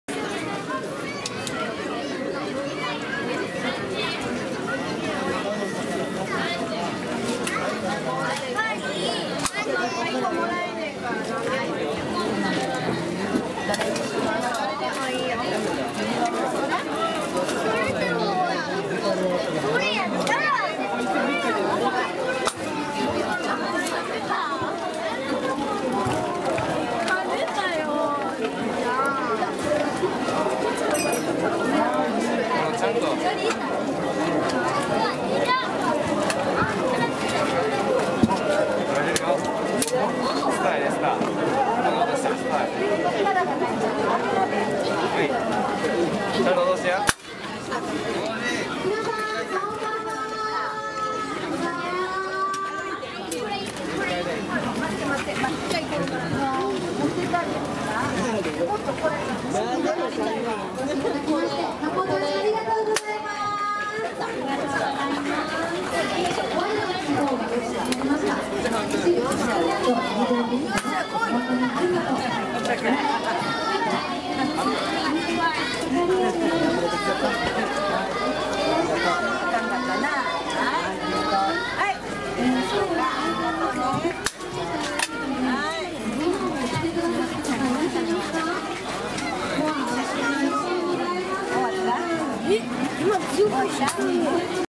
夏の風物詩（金魚すくい）- Summer Festival
日本の縁日では、こうした子供向けの出店が必ずある。
夏は金魚すくいが定番。それとスマートボールや鉄砲で景品を打ち落とすゲームだとか、子供が楽しめるものがある。平和だと実感する。